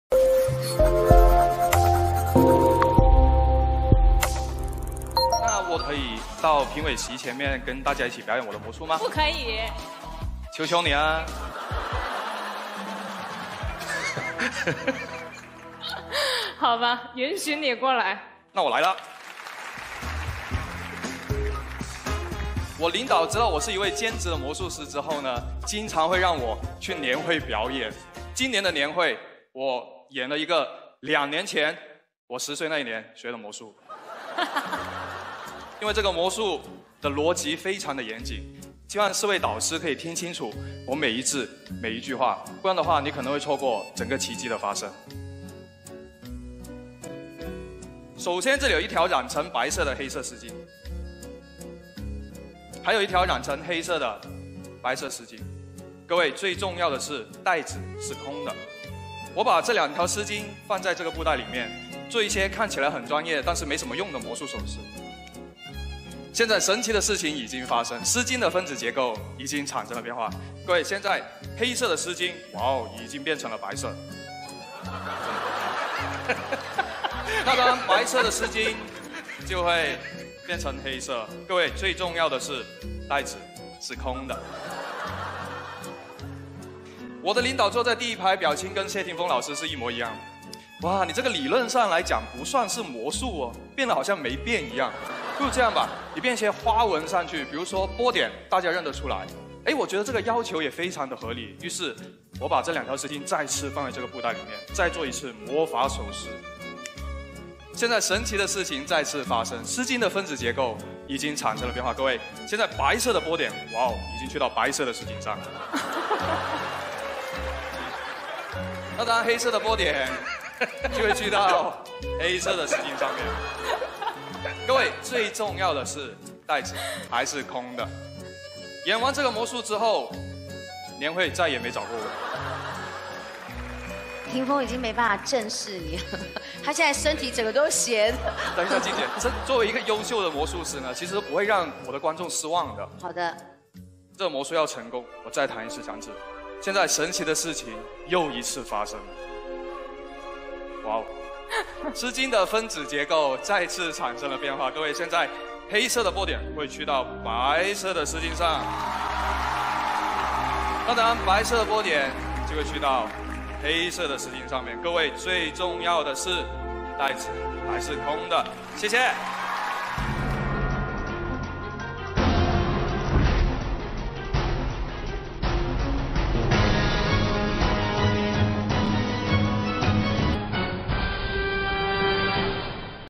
Polka Dot Scarf Change - Magic Trick
Polka Dot Scarf Change

The magician shows a black scarf and a white scarf, and puts them into the bag. With a flick of his fingers, the two scarves change into two scarves with polka dots!

Made of chiffon.

Easy to use. Great for stage and comedy shows.

The scarf measures 40cm*40cm, and the bag measures 15cm in diameter and 30cm in depth.